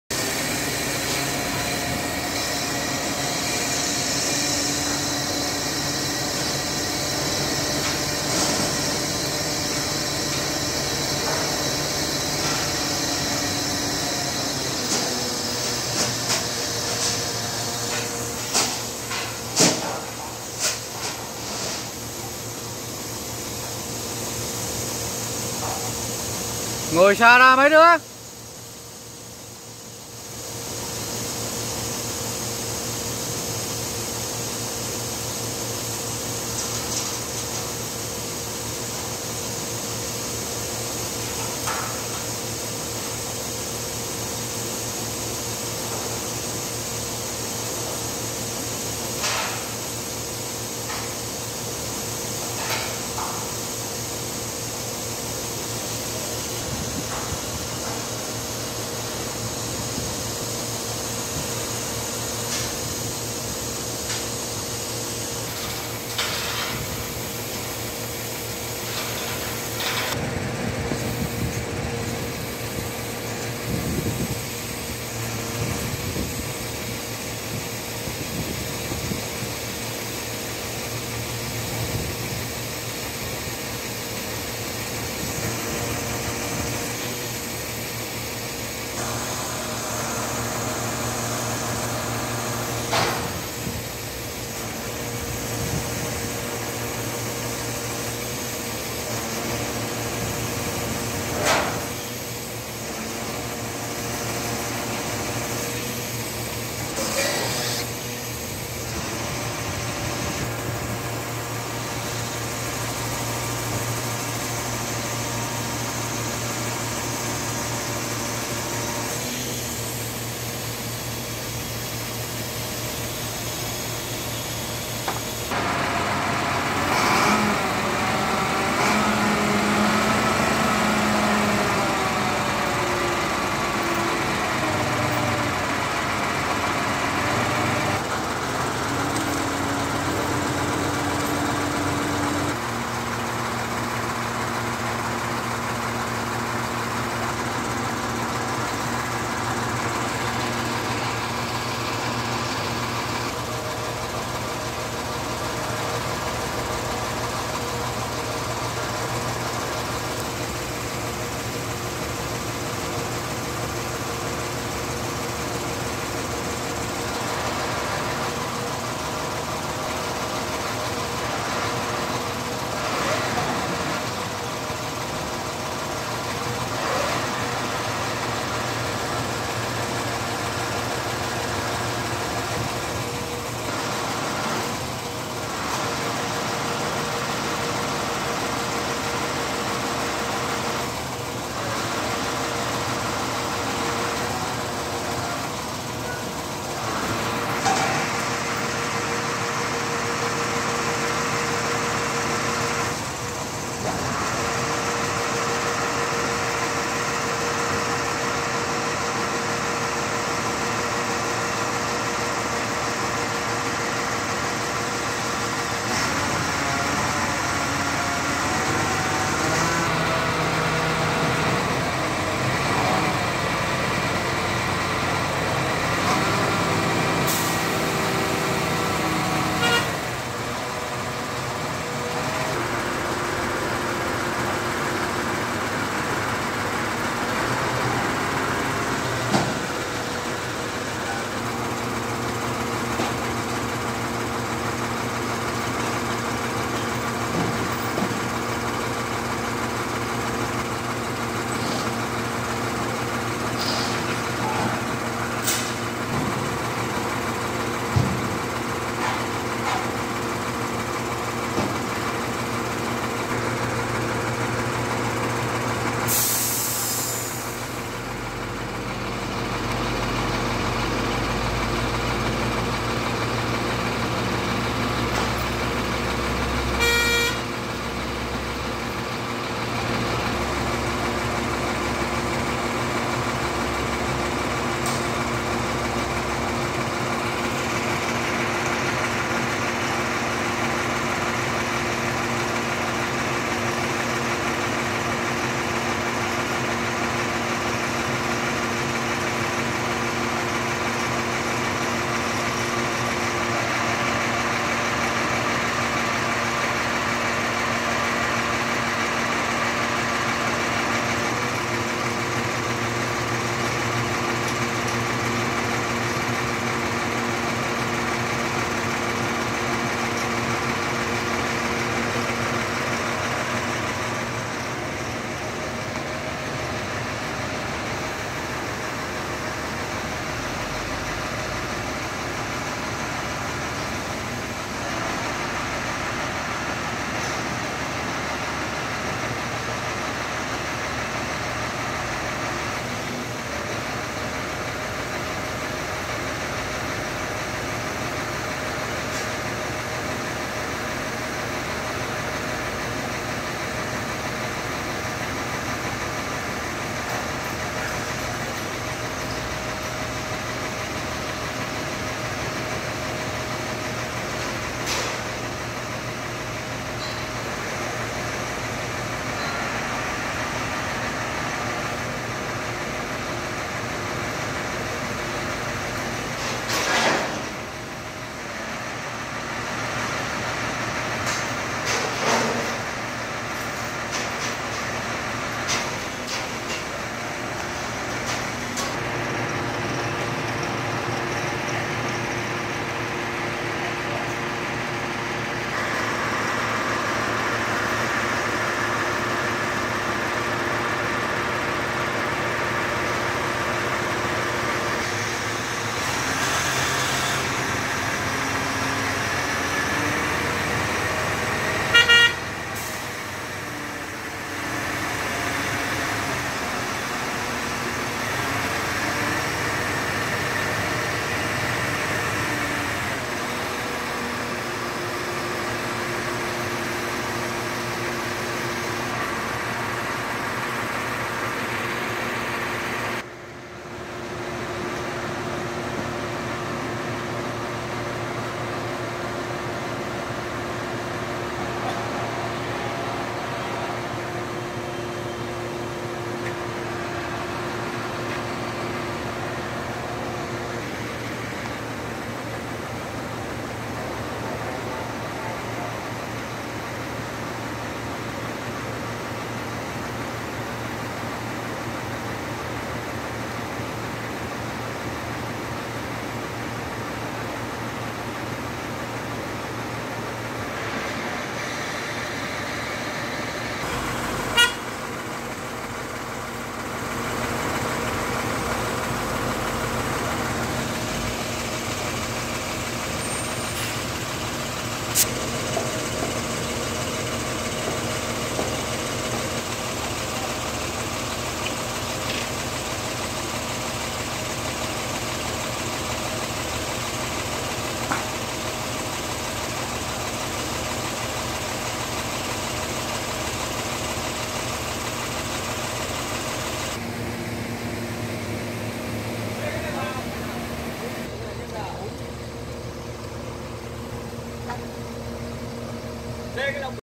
Cách dựng cẩu tháp lên, Tổng kho cẩu tháp Potain Miền Bắc, Tower Crane
Bán và cho thuê cẩu tháp Potain Pháp và vận thăng Alimak thụy điển.